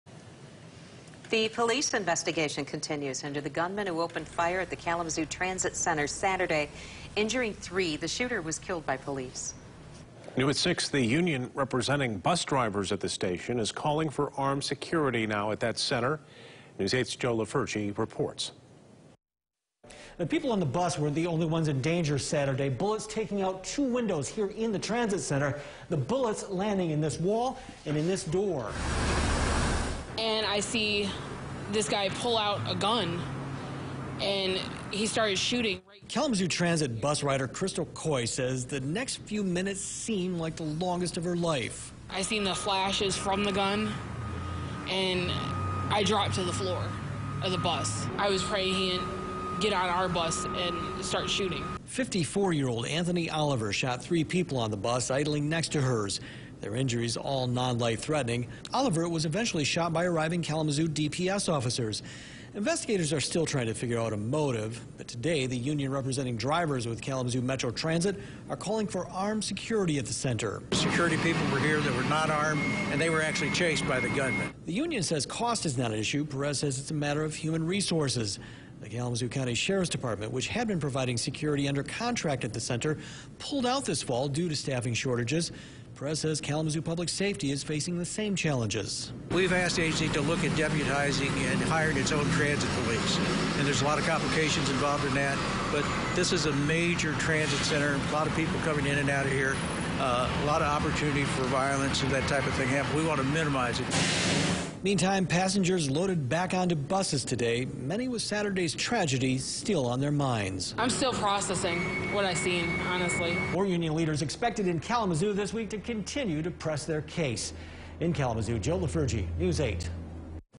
Kalamazoo bus passenger saw 'guy pull out a gun, start shooting'
As the investigation continues into the Saturday morning shooting at Kalamazoo's main bus station that injured three people, wary passengers got back on board and a driver's union called for better protection. (Nov. 29, 2021)